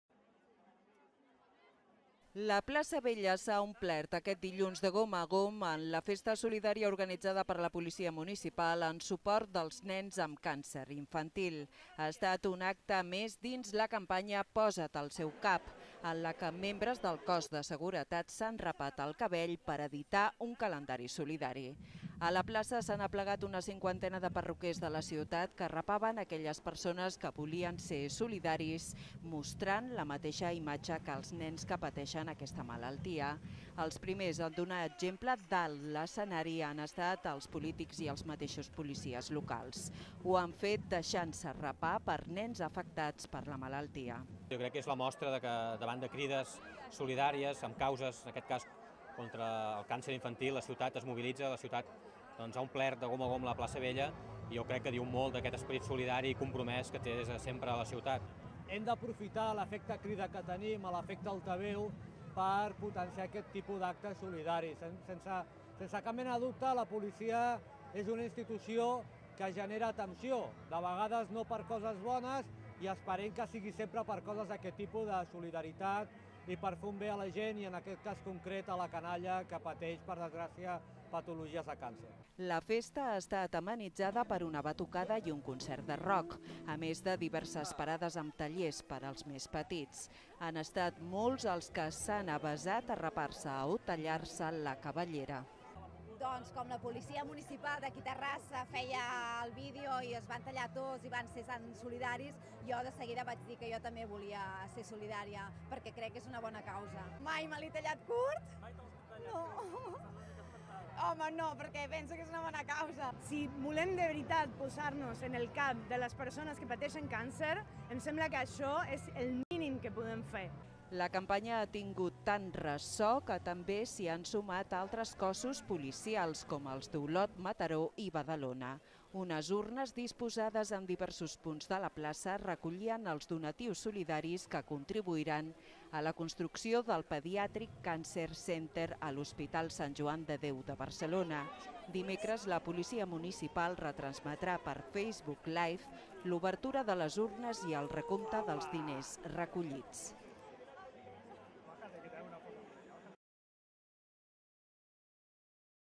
Festa popular en solidaritat amb els infants que pateixen càncer
L'ha organitzat aquest dilluns la Policia Municipal de Terrassa a la plaça Vella. Amb la col·laboració de mig centenar de professionals de les perruqueries de la ciutat, els agents han convidat als i les terrassenques a passar per una macro perruqueria efímera per a rapar-se el cap o tallar-se els cabells, a canvi d’un donatiu per a l’Hospital Sant Joan de Déu de Barcelona, concretament per a la construcció del Pediatric Cancer Center.

A més de la perruqueria, a la plaça s’han instal·lat carpes amb activitats infantils, música en directe, batukada i altres activitats festives.